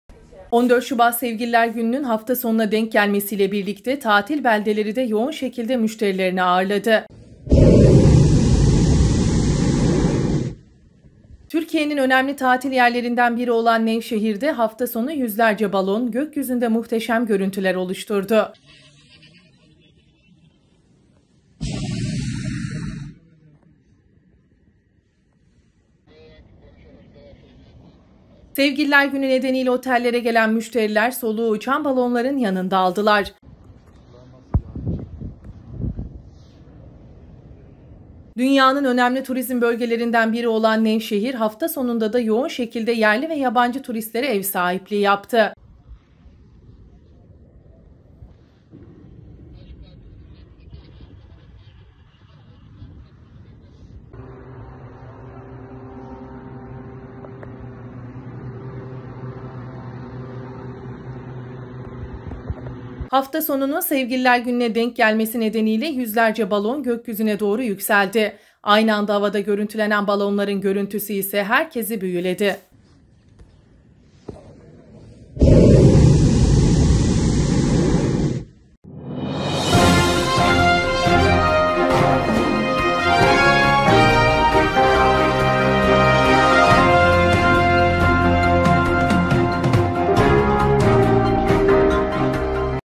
NEVŞEHİR'DE SEVGİLİLER GÜNÜ BALONLAR GÖKYÜZÜNDE...
14 Şubat Sevgililer günü Hatfa sonuna denk gelinci tatil beldeleride yoğun şekilde müşterilerini ağırladı. Türkiye’nin önemli tatil yerlerinden biri olan Nevşehir’de hafta sonu yüzlerce balon gök yüzünde muhteşem görüntüler oluşturdu.